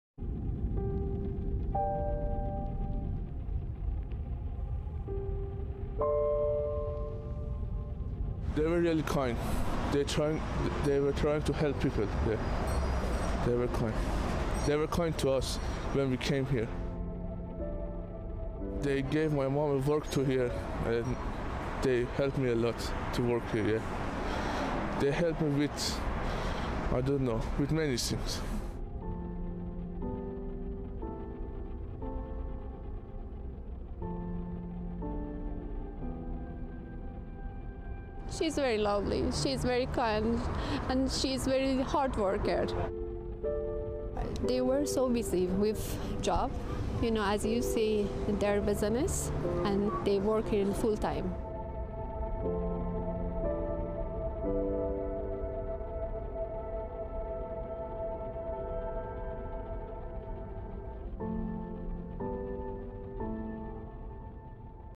North Vancouver's Iranian community remembers mother, daughter killed in UIA Flight 752 crash
When Flight 752 was shot down in Iran, killing all on board, one of the focal points for those grieving in Vancouver became Amir bakery in North Vancouver. Mourners gathered in front, lighting candles, and praying. Owner Amir Pasavand lost both his daughter Fatemah Pasavand and his wife Ayeshe Pourghaderi. This is how they're being remembered.